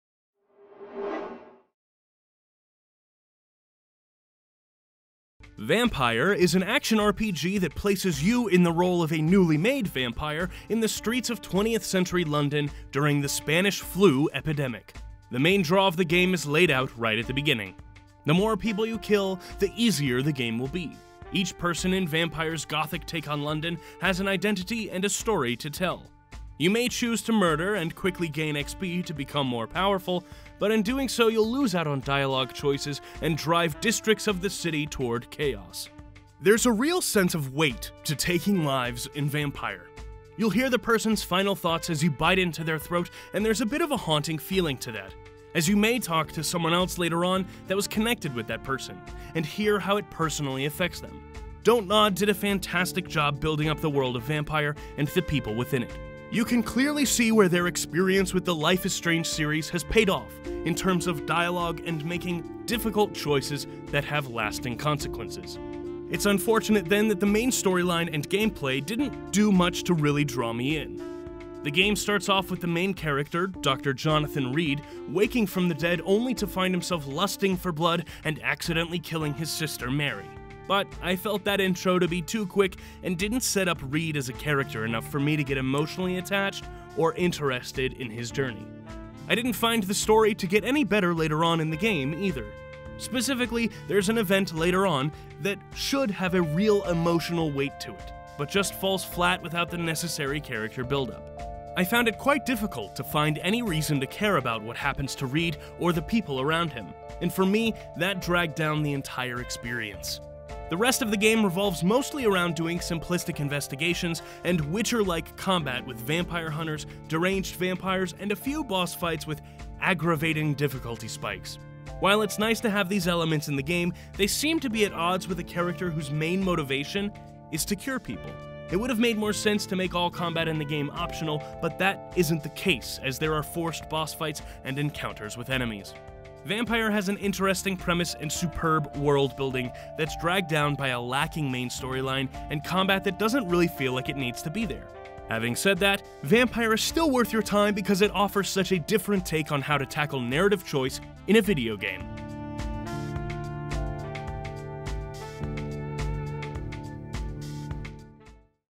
Vampyr | Review in 2 Minutes
Nick Calandra reviews the latest game from Dontnod, the developers of Life is Strange.

Follow Gameumentary For More!